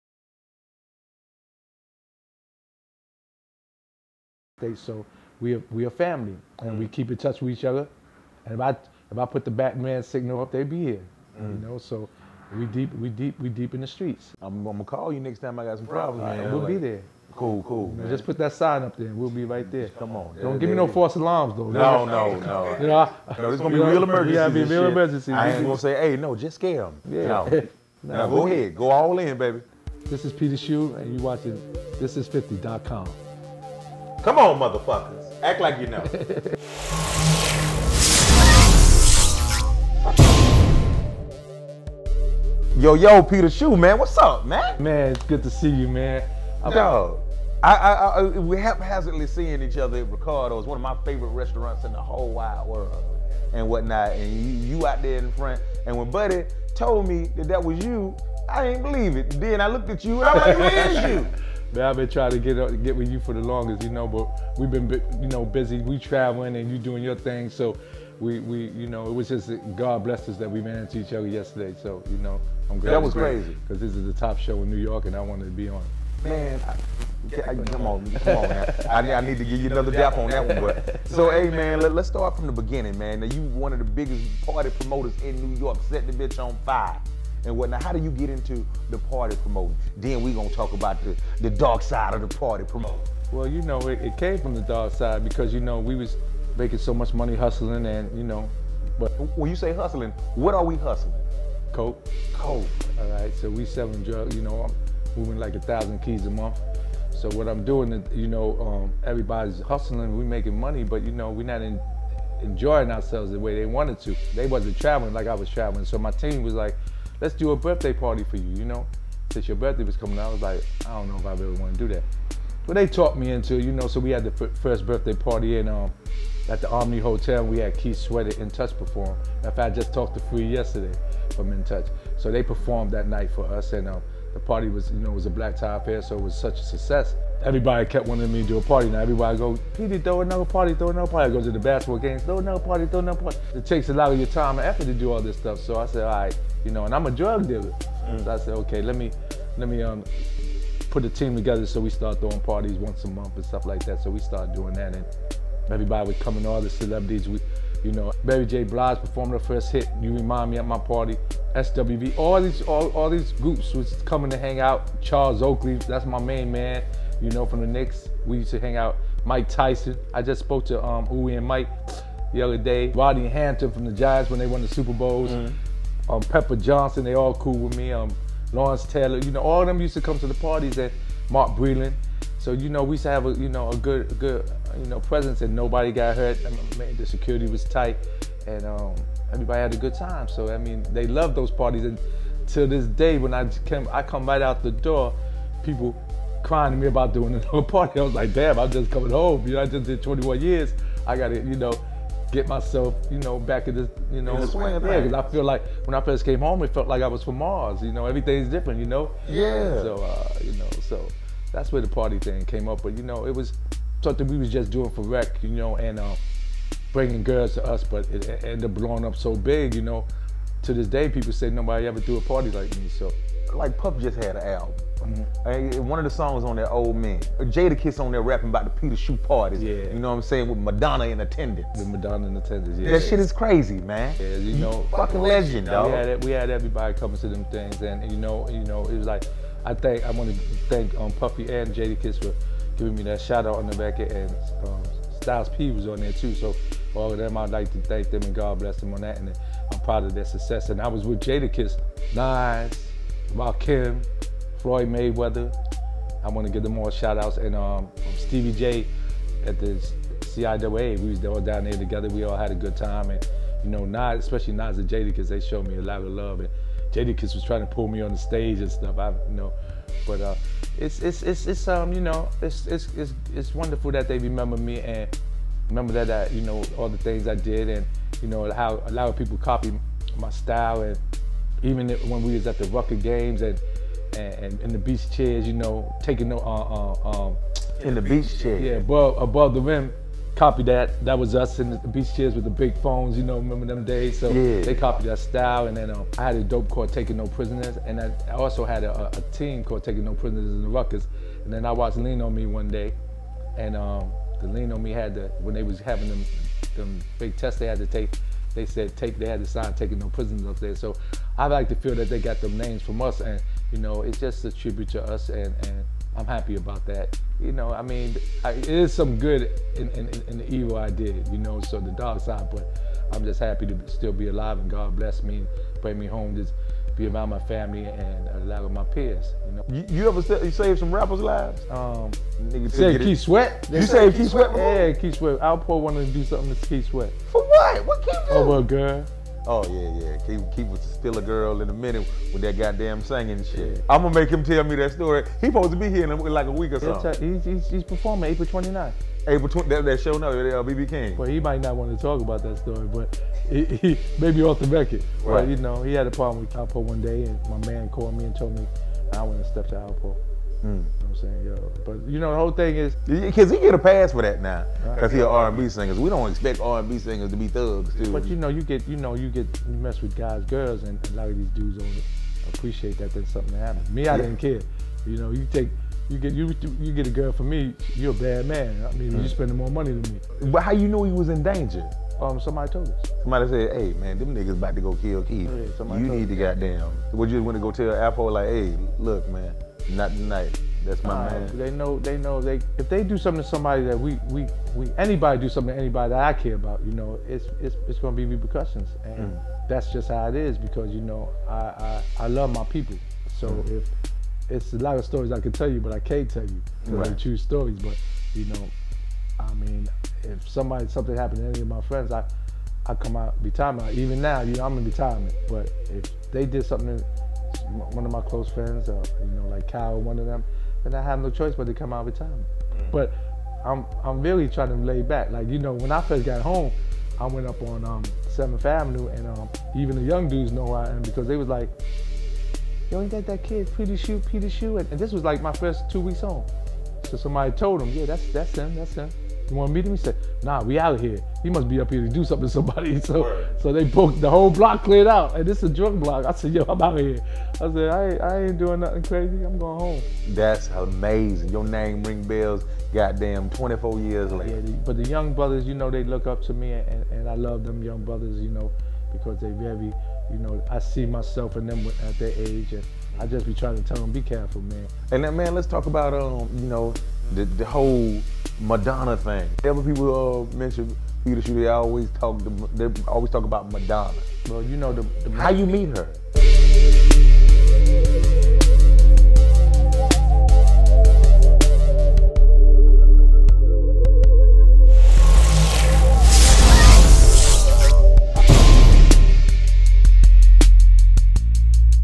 Club King Peter Shue on Hustling; Saving Keith Sweat's Life ; 21 Years in Prison
Thisis50 & Young Jack Thriller recently spoke with Peter Shue for an exclusive interview!

Club King Peter Shue son Hustling, Saving Keith Sweat's Life, 21 Years in Prison & much more!